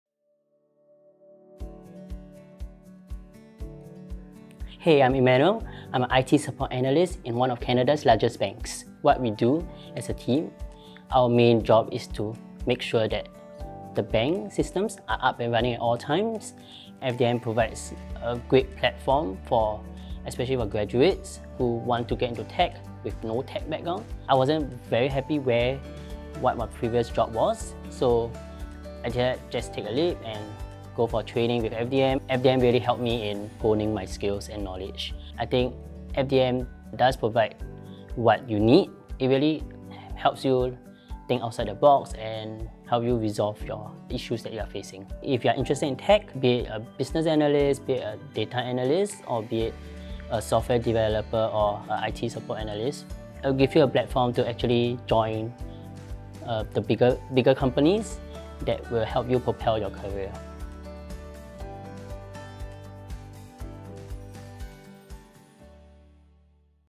Non-STEM Grad to Tech Consultant: Emmanuel's FDM Consultant Story
Join Emmanuel Ho on his journey from university graduate to FDM Consultant working as an IT Support Analyst. Learn how FDM helped Emmanuel start a tech career despite not having a STEM degree. Discover how FDM helped him develop his skills, knowledge, and ability to think outside the box to solve challenges.

Find out more about our graduate career opportunities by clicking on this